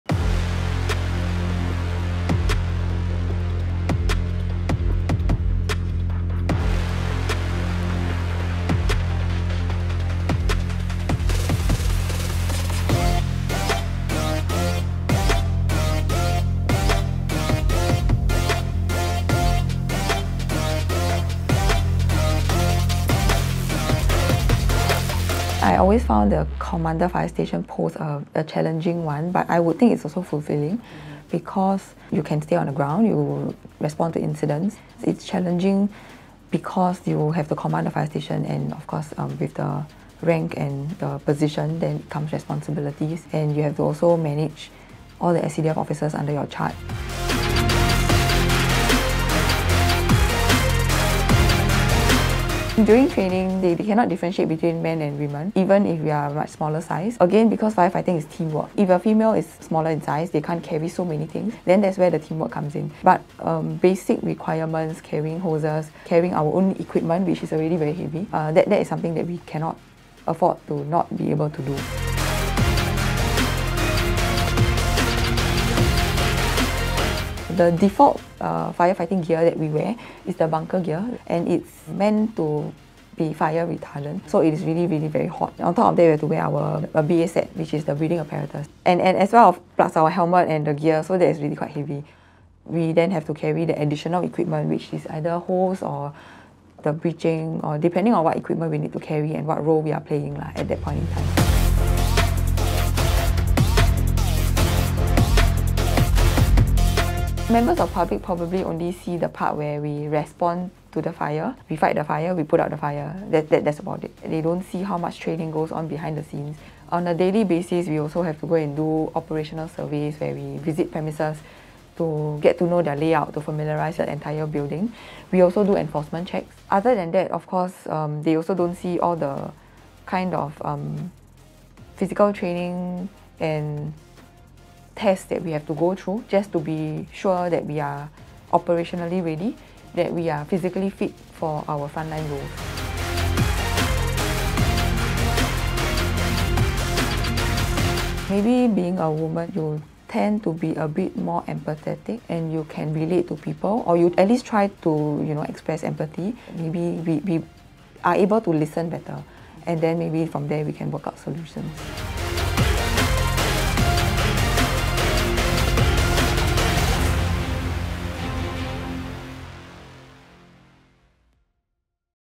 The woman in charge at Sentosa Fire Station: Lieutenant Colonel Jennyline Fan
After about a decade in public service, Lieutenant Colonel Jennyline Fan is now commander of the Sentosa Fire Station. The 34-year-old tells CNA Women what drew her to a career with the Singapore Civil Defence Force – and what it was like to train with up to 60kg of gear and equipment on her body.